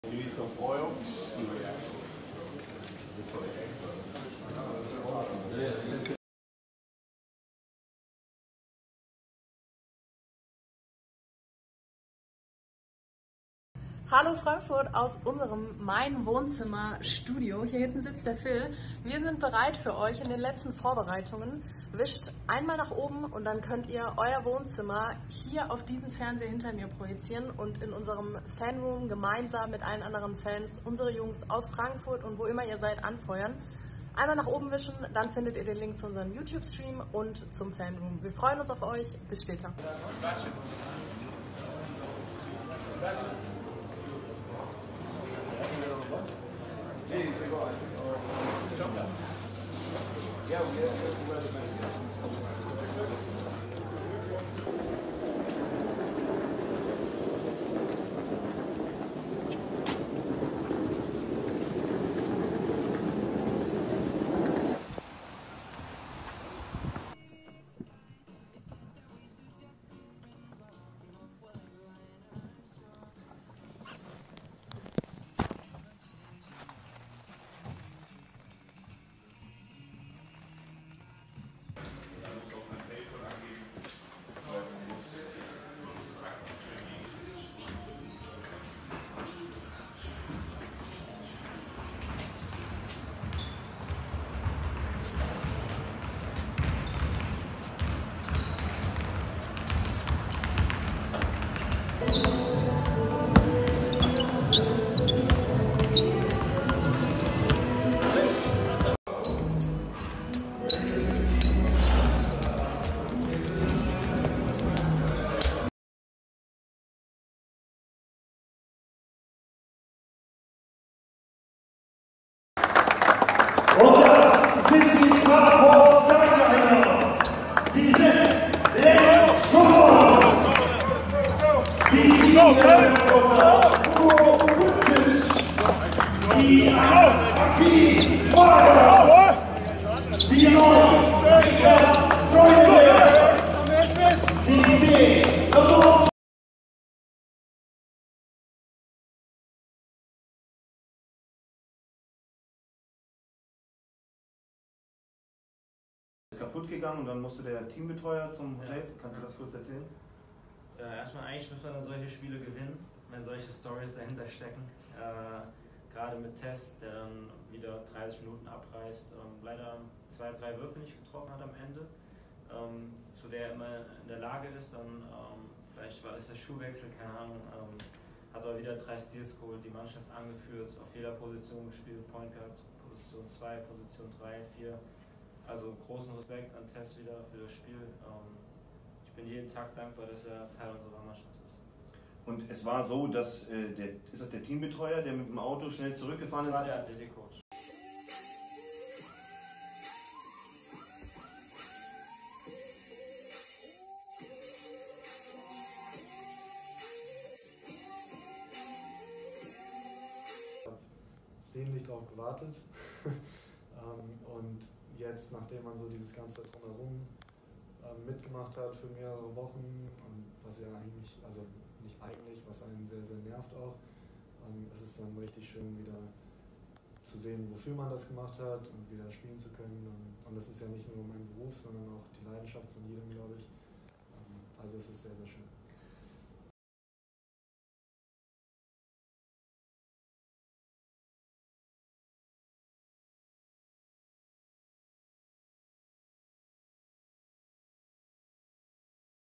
Tagebuch Teil 3
Die Instagram-Story als Video. Einblicke, HIntergründe, garniert mit Witz, Charme und jeder Menge FRAPORT SKYLINERS Innenleben.